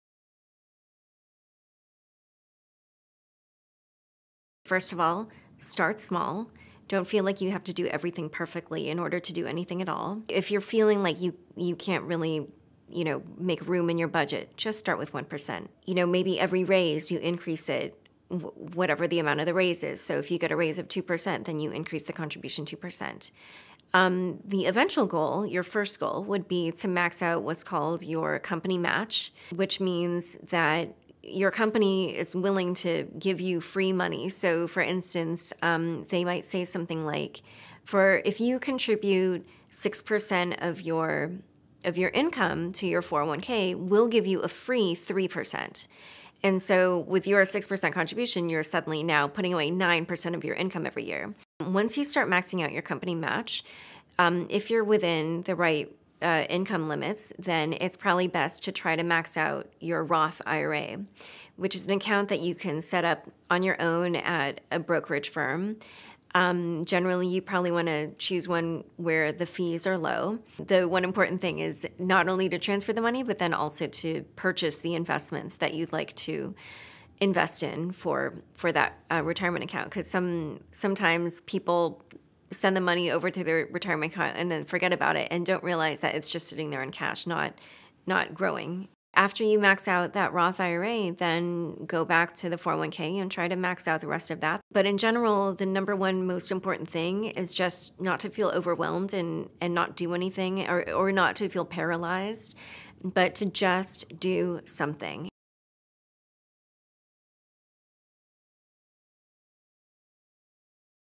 How to Start Financial Planning | When I'm 65
Don't know where to start when it comes to saving for retirement? Laura Shin, financial journalist with Forbes, shares some easy first steps.